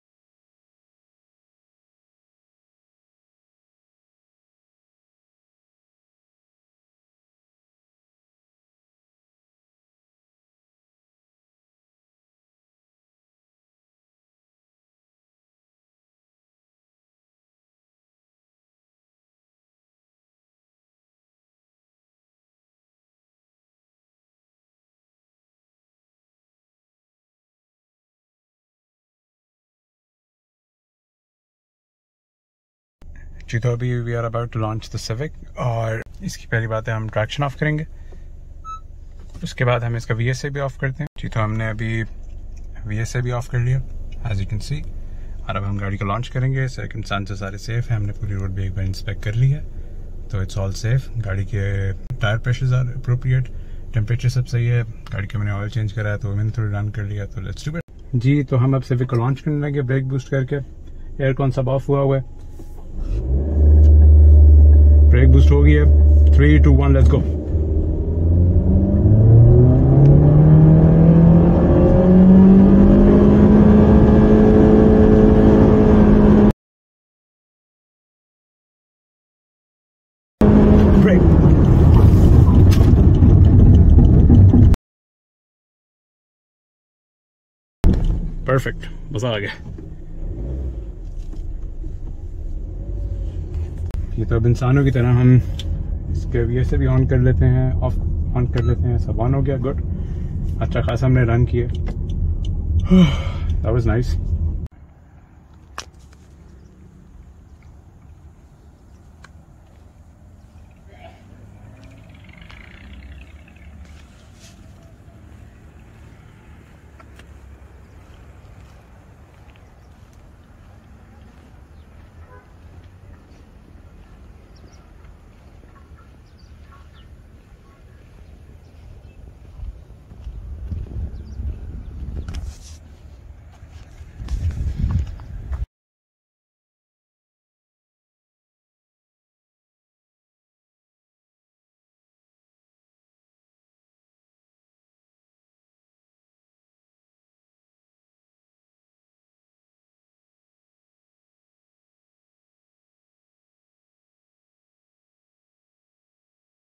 0 - 100 Km/h & Brake Test | Honda Civic UG 10th Generation | Pakistan | 4K
0 - 100 Km/h & Brake Test | Honda Civic UG 10th Generation | Pakistan | 4K.
Such run executed under safe and sound circumstances.